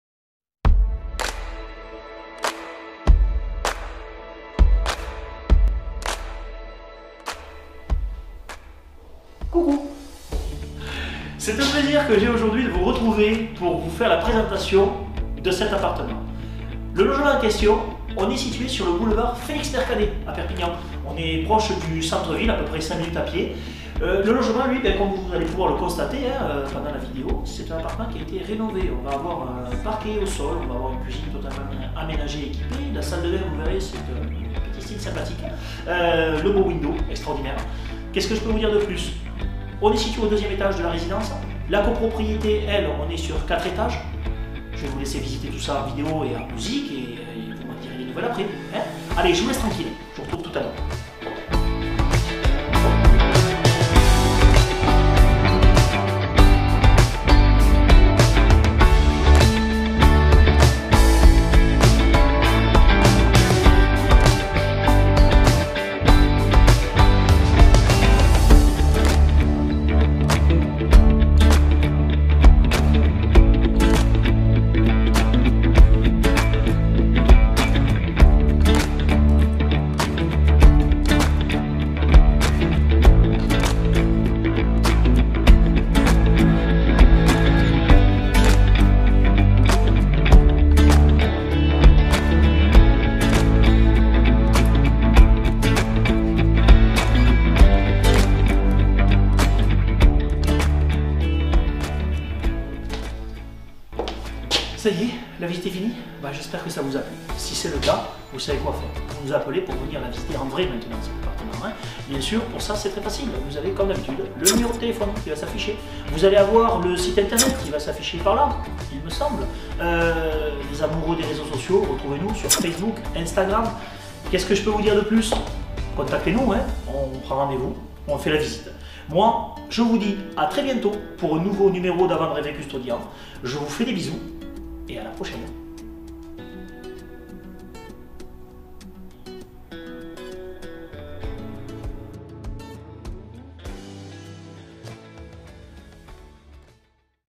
beaux volumes pour cet appartement de type T2 à proximité du centre ville de Perpignan
Avec l'agence Custodian, venez découvrir ce superbe appartement de type T2/3.

Sur notre site internet vous découvrirez :

- Une vidéo de présentation de l'appartement
- Une visite virtuelle 360° HD permettant même de prendre des mesures
- Des photos HD

Sur le Boulevard Félix Mercader, cet appartement est situé au 2ème étage de la résidence.
Vous découvrirez en passant la porte, une très belle pièce de vie lumineuse, avec son bow-window!
Entièrement rénové, avec parquet, menuiseries avec double vitrage phonique, volets roulants électriques.
Vous prendrez plaisir à concocter de bons petits (et grands) plats, dans cette jolie cuisine, aménagée et équipée.
La chambre grande et lumineuse s'ouvre sur un joli balcon.
Salle de bains, wc séparés et au sous sol, une cave.

Alors n'hésitez plus, contactez-nous pour convenir d'une visite.